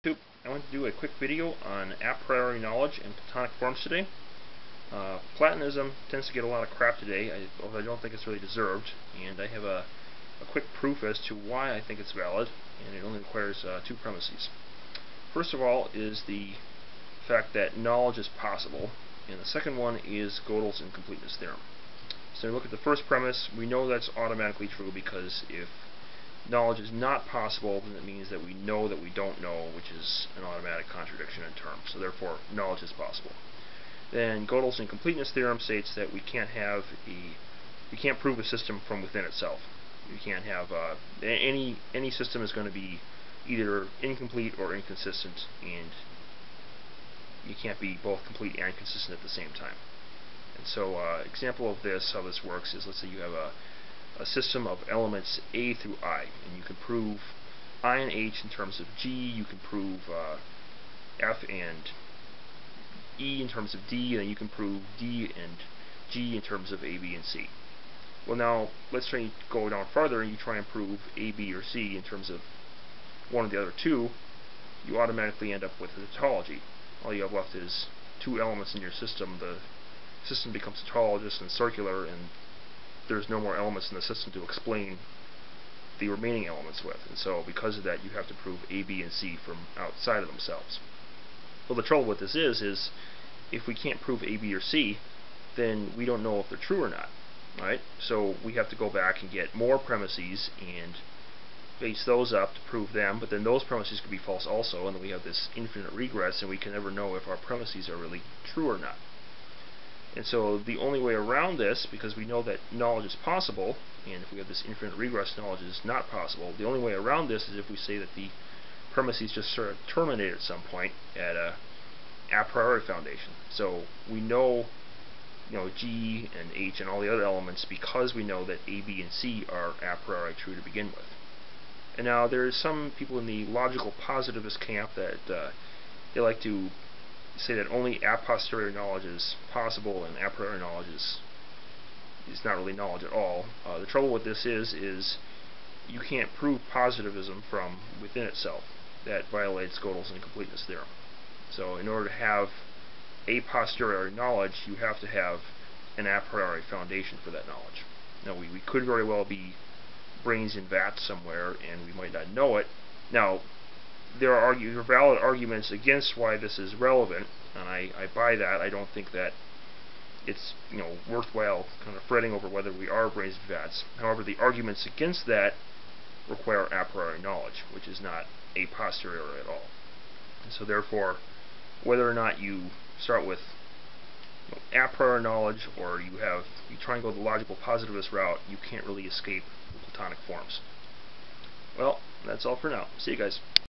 Why Platonic Forms Exist: A Simple Proof from Godel's Theorem
The notion of Platonic forms often gets a bad rap these days, however without it no true knowledge would be possible.

Godel's incompleteness theorem tells us that no logical system can be complete and consistent at the same time. To prove something we need premises which themselves are unproved from within that system.

However this leads us to an infinite regress, wherein no knowledge is possible. Knowledge by definition is justified true belief and if we can't completely prove something then we can not completely justify it and therefore truly know it. Thus knowledge must be grounded on something innate at some level -ie. Platonic forms or the a priori category.

It has been argued by positivists that only the a posteriori category is necessary. However this is self-refuting. A posteriori knowledge can not be justified without reference to a priori knowledge. As such denying a priori knowledge while accepting a posteriori knowledge commits the Stolen Concept Fallacy. It accepts a posteriori knowledge while rejecting the basis of that knowledge.

On a side note; Platonism does not equal mysticism as is often erroneously suggested by some of its detractors. A variant of Platonism called Neoplatonism often does include mystical elements, however this is not necessary for Platonist epistemology in and of itself.